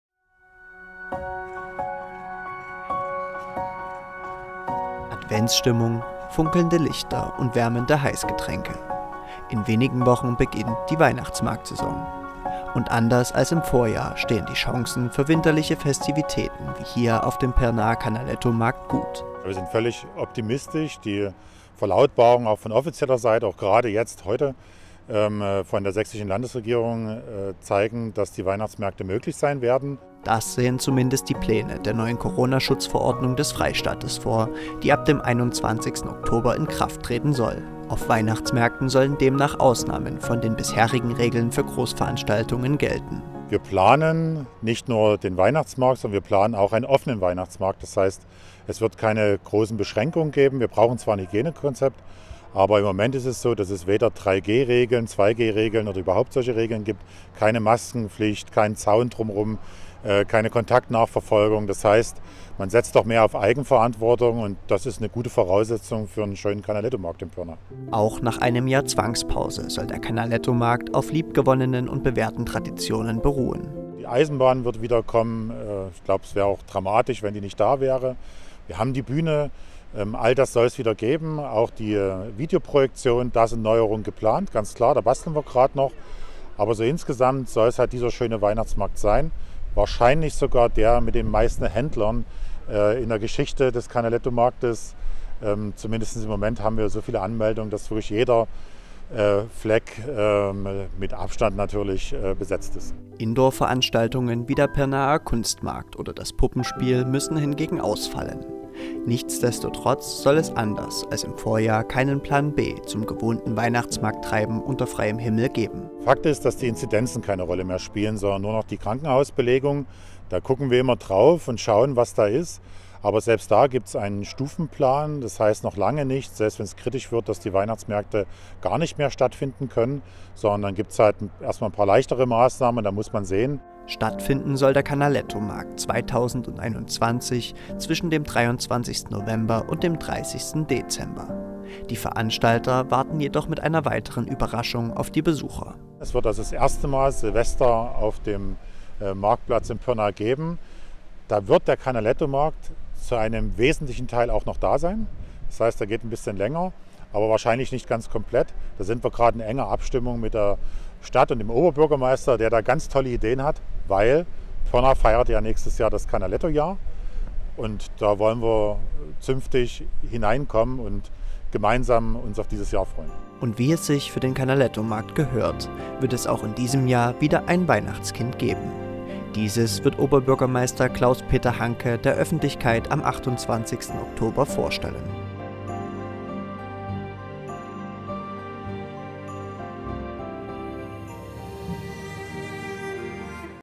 Nach einem Jahr Zwangspause: Canalettomarkt steht in den Startlöchern!
Stattfinden soll der Canalettomarkt 2021 vom 23. November bis 30. Dezember. Entsprechend der neuen Corona-Schutzverordnung sogar ohne 2/3-G-Regelung. Über die Hintergründe und den Planungsstand informiert Betreiber Holger Zastrow.